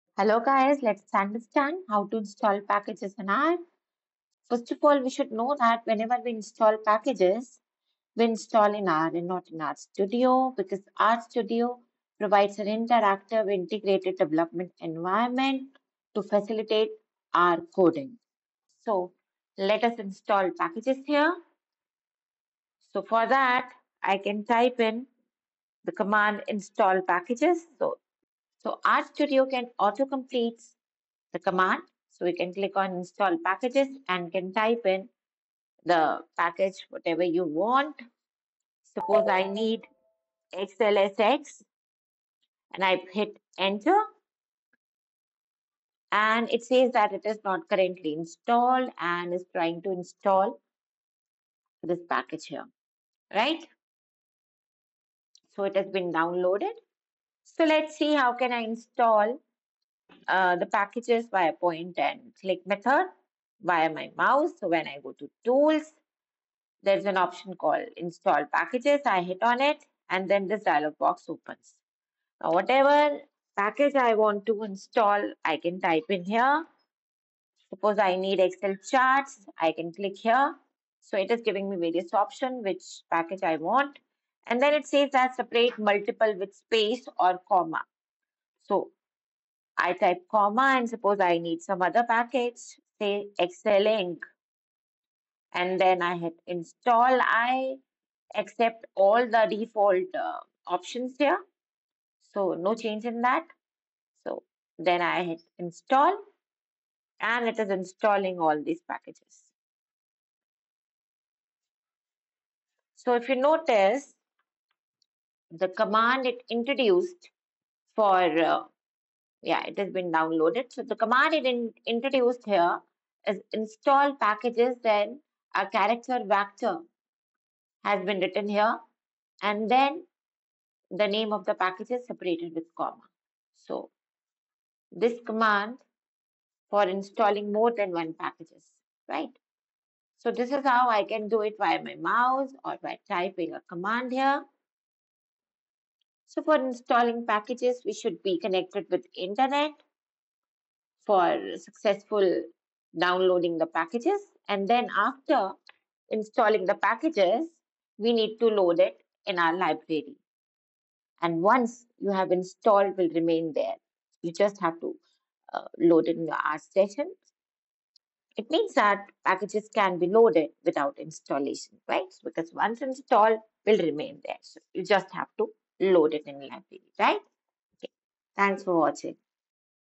R Programming : How to Install Packages in R Studio
In this tutorial  @anitamendir , I'll guide you through the simple and efficient process of installing packages in R Studio. Learning how to install packages in R is essential for expanding your programming toolkit.